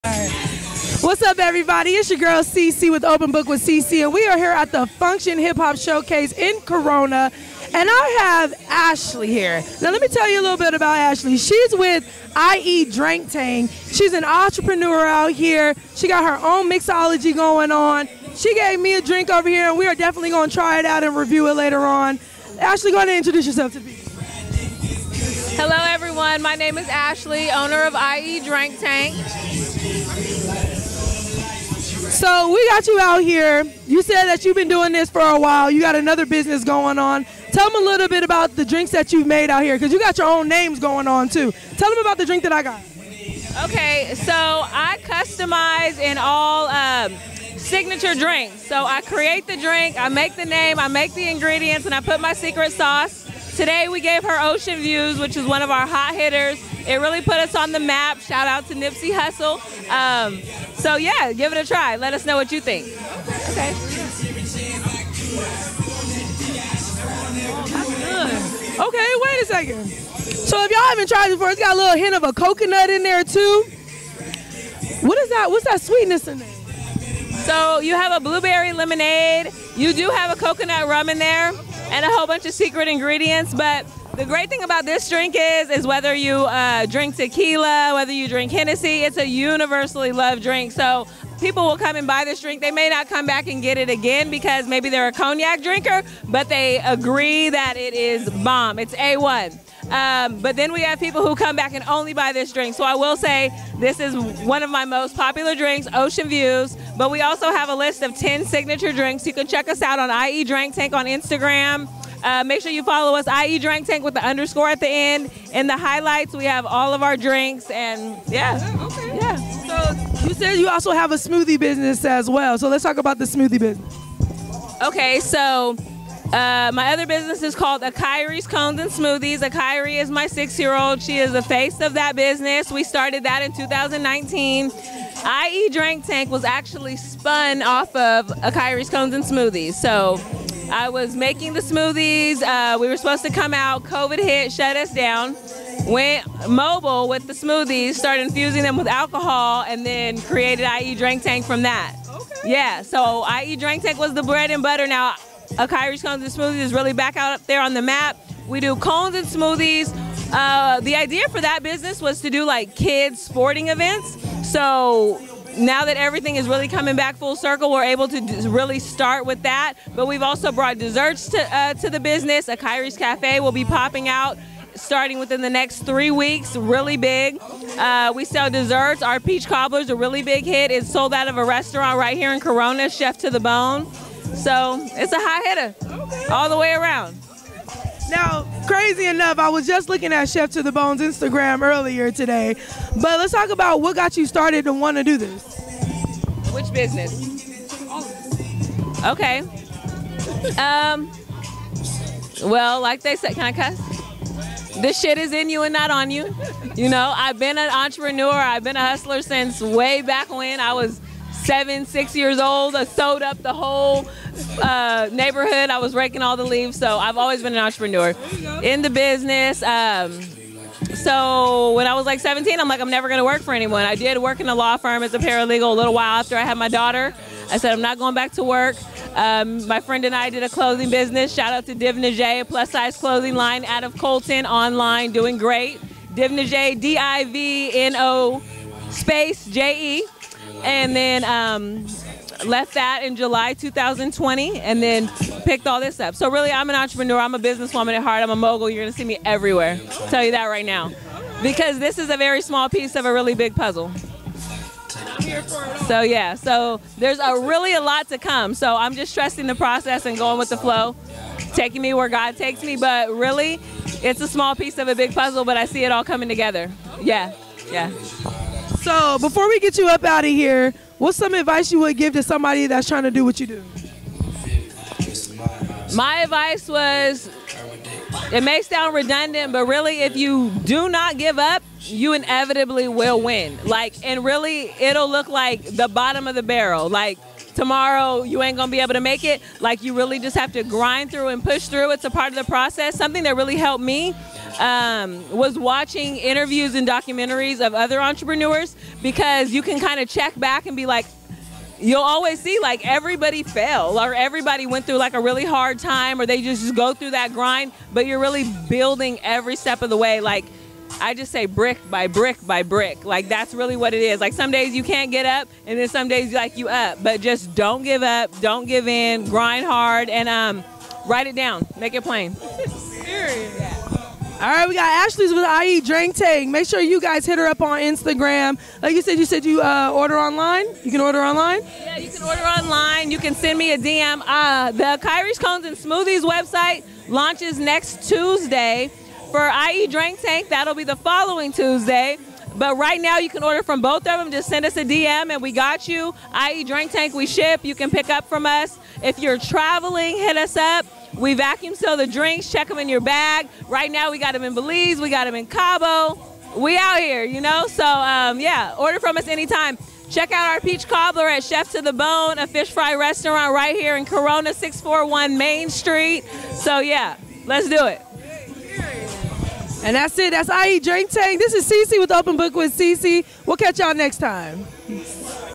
Ashley of IE Drank Tank gave us an insight of her business and her famous Nipsey approved drink.
Make sure yall book IEDrankTank fornyour nexr business vendor!!